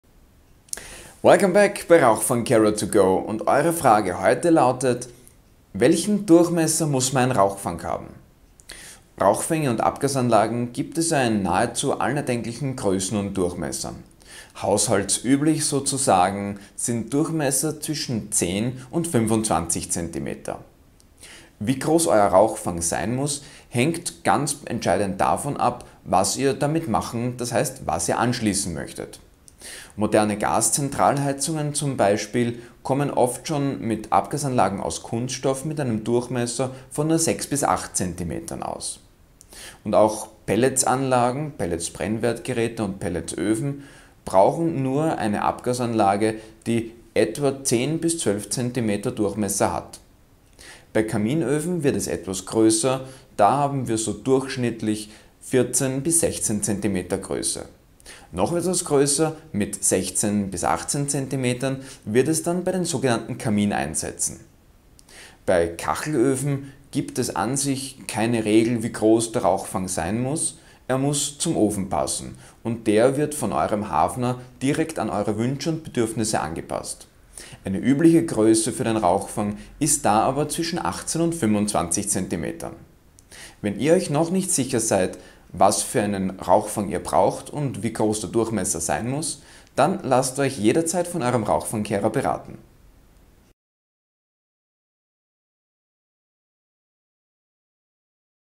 Welchen Durchmesser muss mein Rauchfang haben? - Rauchfangcarer2Go, Folge 39
Bevor der Hut brennt!

Bei Rauchfangcarer2Go geht es diesmal darum, welcher Durchmesser euer Rauchfang haben muss.

Der Durchmesser hängt vom Anwendungsfall an:

Moderne Gas-Zentralheizungen haben Abgasanlagen aus Kunststoffen mit einem Durchmesser von Ø 6-8 cm.

Pelletskessel- und öfen arbeiten üblicherweise mit Abgasanlagen mit 8-12 cm.

Bei Kaminöfen wird es mit Ø 14-16 cm etwas größer.

Ø 16-18 cm sind der Standard bei Kamineinsätzen. Große Einsätze mit stärkerer Heizleistung können auch einen Rauchfang mit Ø 20 cm brauchen.

Für Kachelöfen gibt es keine Grenzen, da sie von eurem Hafner nach euren Wünschen maßgeschneidert werden. Übliche Rauchfangdurchmesser sind aber Ø 18-25 cm.

Falls ihr unsicher seid, was ihr vielleicht einmal anschließen möchtet, ist ein Rauchfang mit Ø 18-20 cm Durchmesser eine gute Wahl. Damit seid ihr flexibel und könnt alle gängigen Geräte anschließen. Falls dieser Durchmesser dann zu groß ist, kann immer noch z.B. eine Abgasanlage aus Kunststoff eingezogen werden.